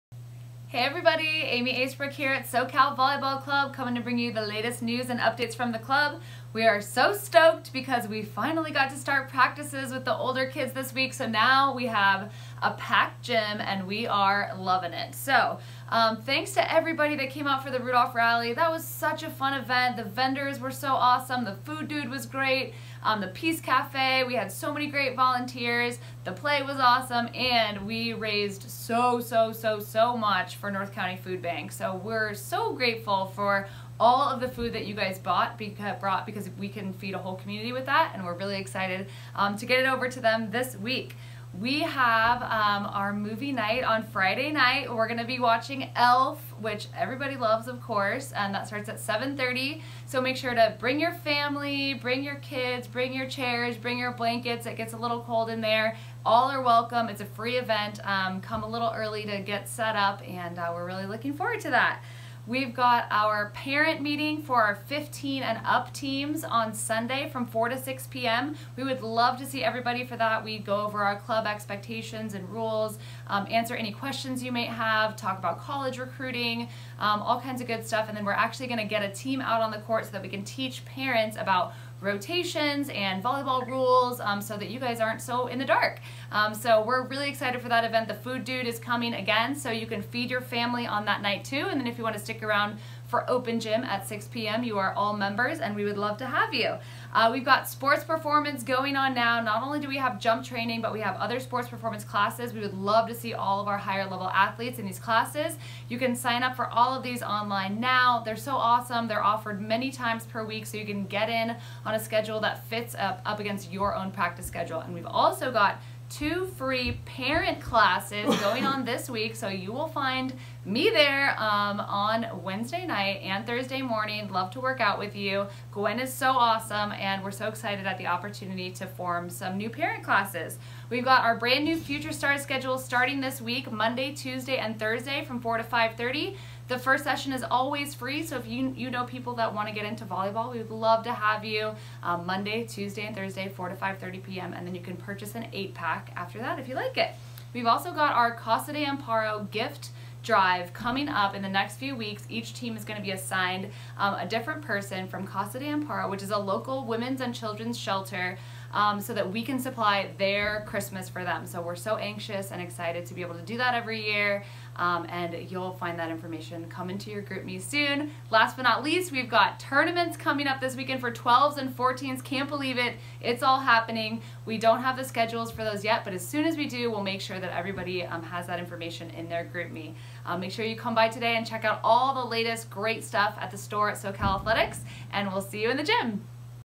This Week at SoCal with Aimee Asebroek - 11/27/18
Links to everything Aimee is talking about:

-- Movie Night: Friday, November 30 at 7:00pm

-- Parent Night: Sunday, December 2 at 4:00pm